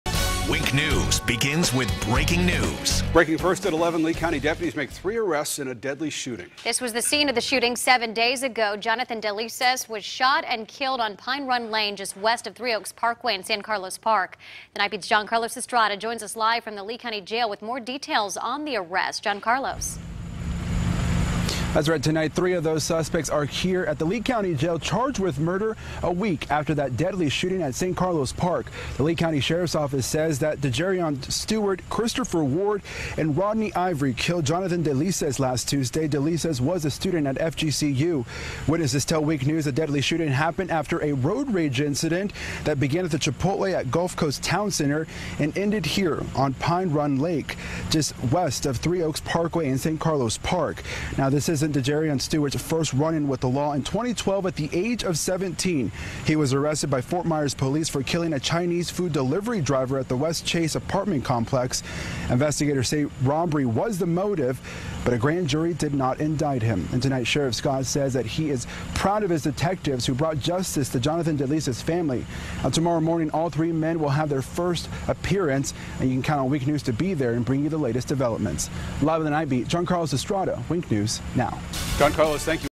Three men arrested in San Carlos Park homicide
WINK News Anchor John-Carlos Estrada has breaking news on the arrested of three men in the killing last week in San Carlos Park. (July 19, 2017)